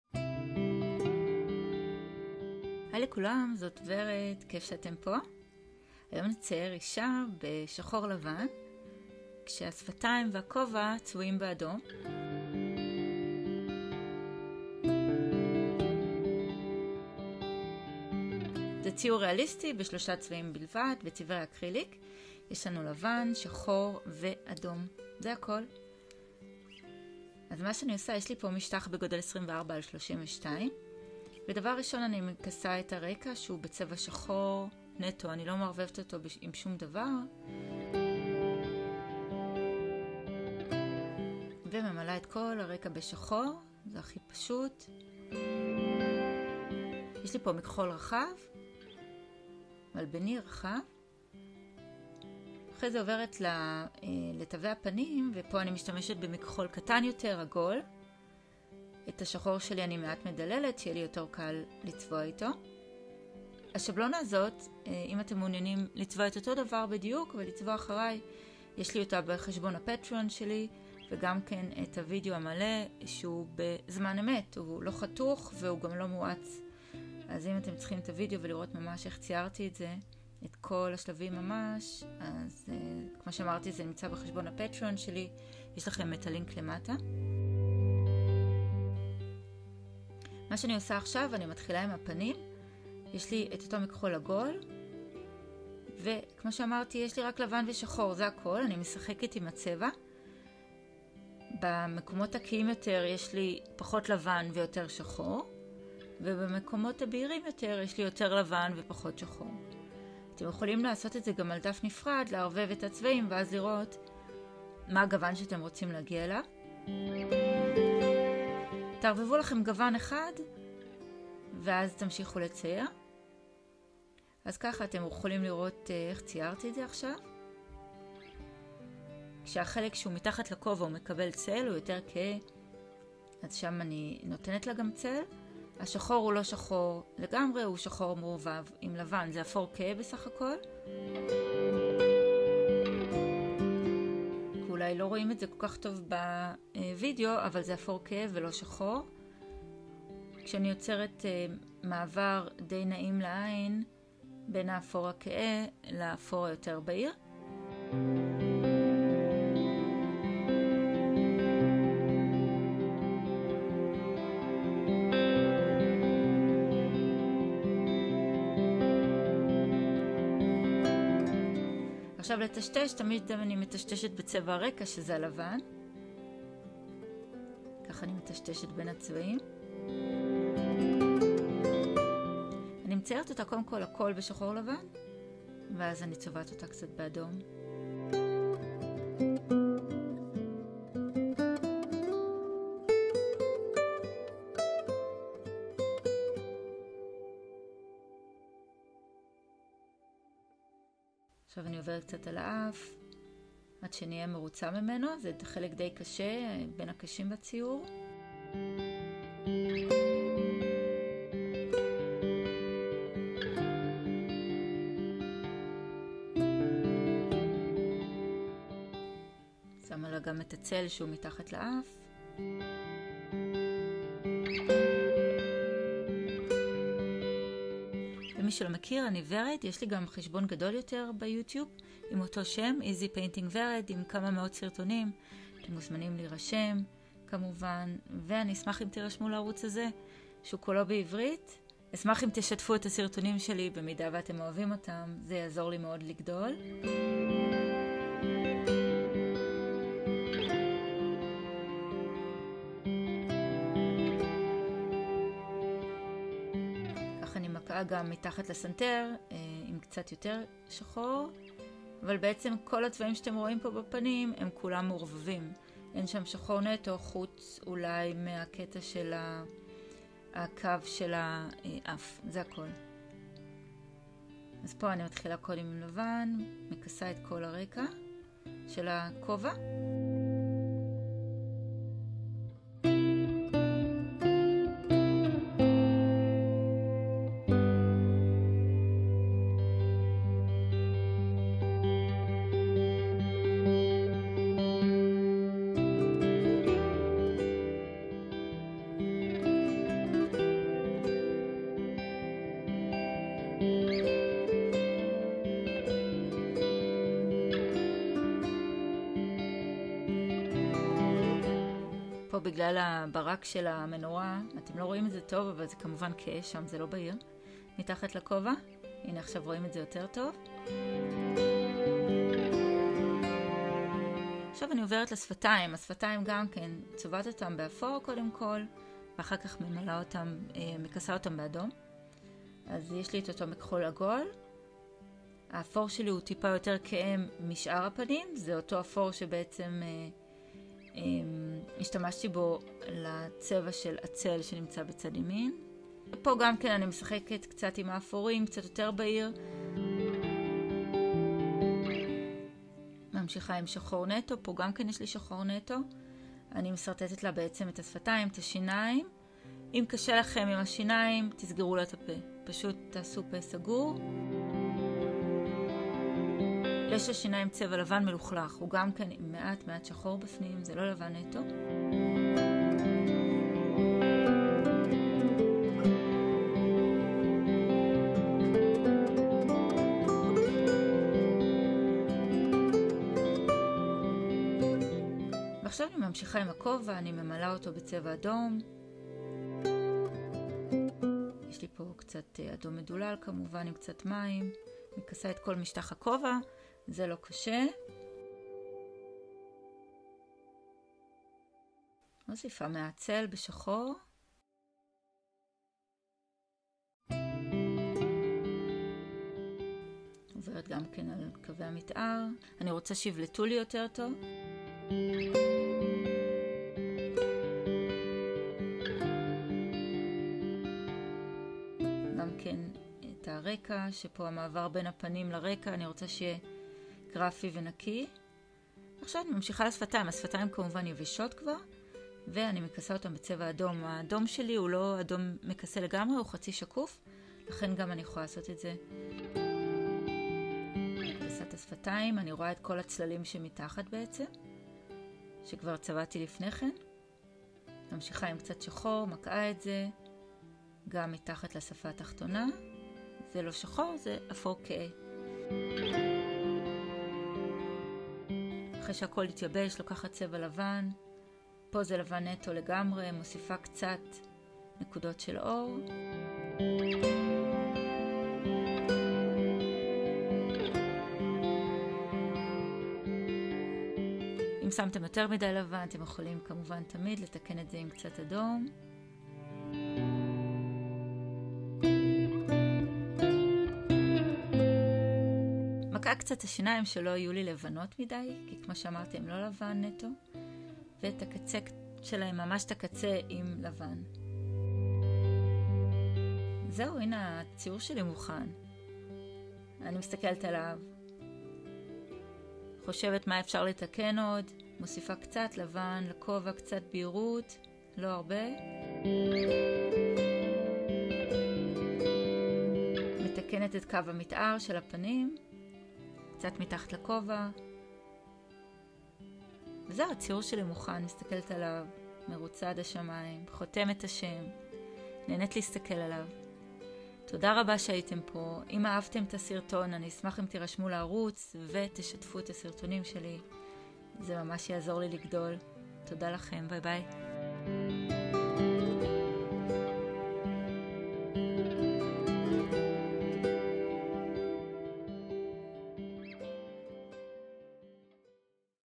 איך לצייר על קנבס - אישה באדום / שיעור ציור באקריליק עם הסבר בעברית
ציור על קנבס באקריליק למתחילים/ שיעור ציור בעברית/ מציירים ביחד אישה עם כובע אדום
היי לכולם, סרטון המראה איך לצייר את לצייר בצבעי אקריליק.
אהבתם? אשמח אם תשתפו, תרשמו לערוץ ותעשו לי לייק.

לערוץ הראשי שלי עם יותר מ270 סרטונים (באנגלית)

לסרטון המסביר איך להעביר ציור לדף

אינסטגרם